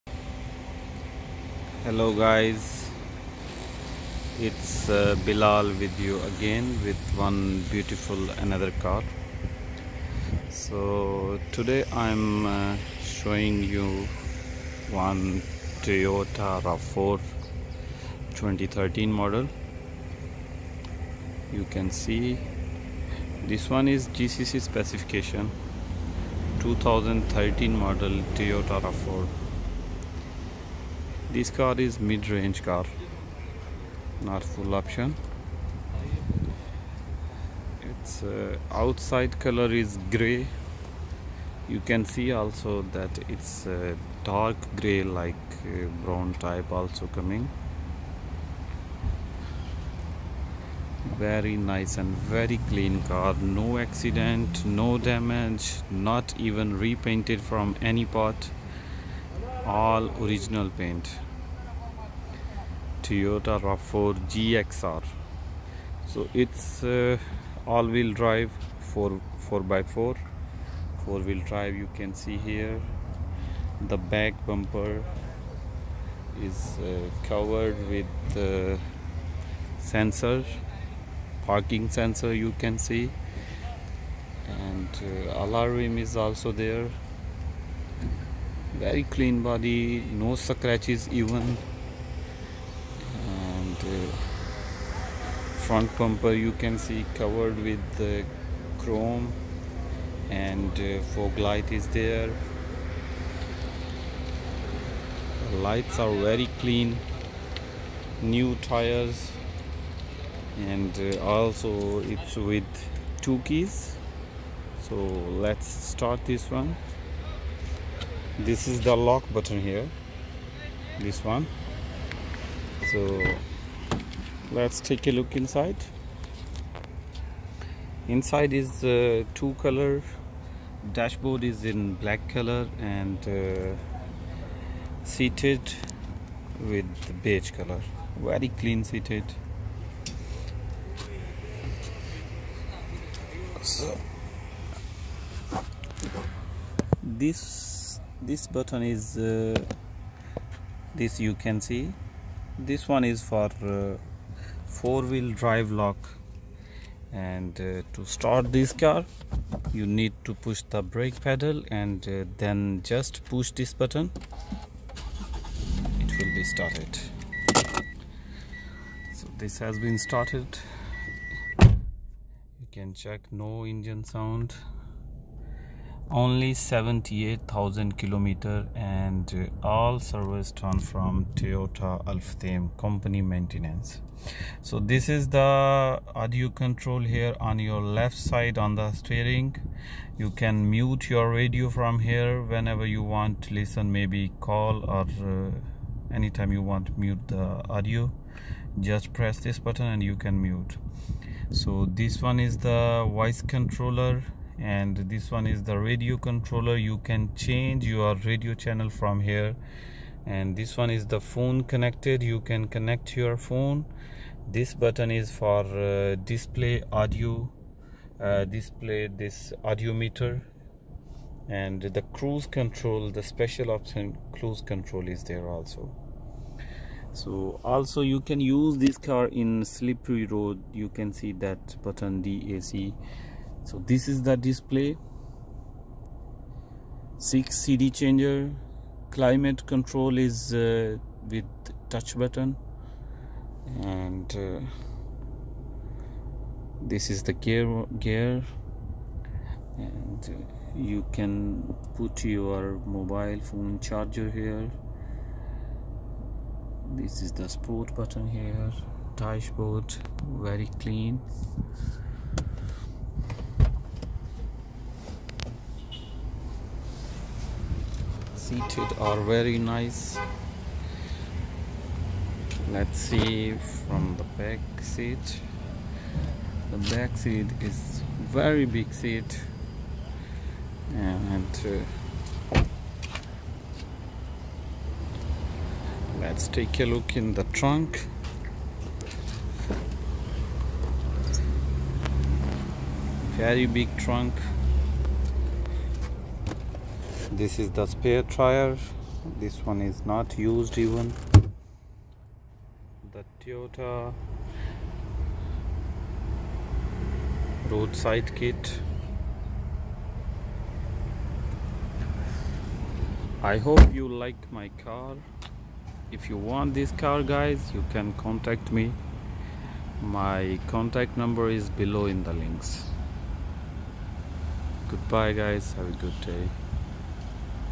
Toyota RAV4 2013 GXR GCC .. تویوتا رافور ٢٠١٣ خلیجی ۔۔۔۔
rav4 2013 GXR with ±push start engine ±4WD ± criuse control ± fog lights
Only 78000 KM

Car located in
Sharjah used car market ... souq al haraj